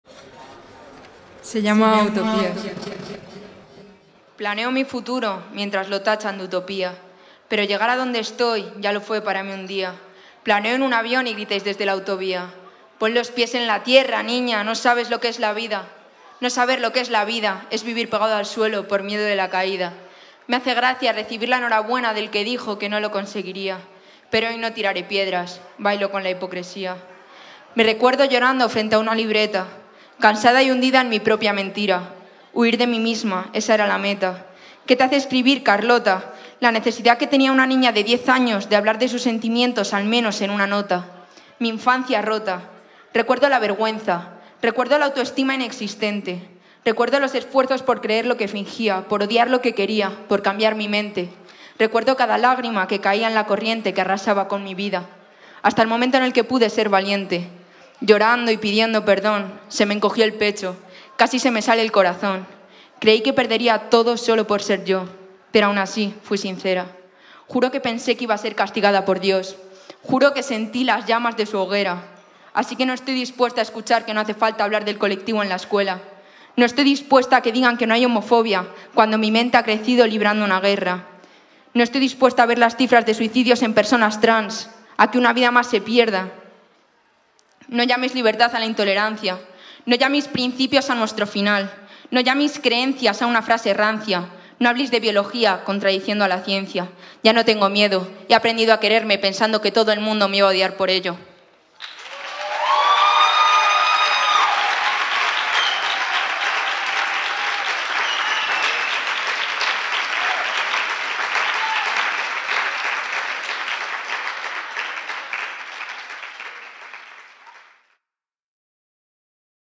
Utopías - Carlota Erroz ____Final Poetry Slam Escaparate 2020
Final de Poetry Slam Escaparate 2020. Primera ronda.

Carlota, campeona de Poetry Slam Online 2020 a través del Instagram de Escaparate de Poesía, se abría así en un escenario.

Recita: Utopías

Carlota en Instagram: @carlotaerroz
Escaparate de Poesía en Instagram: @escaparatedepoesia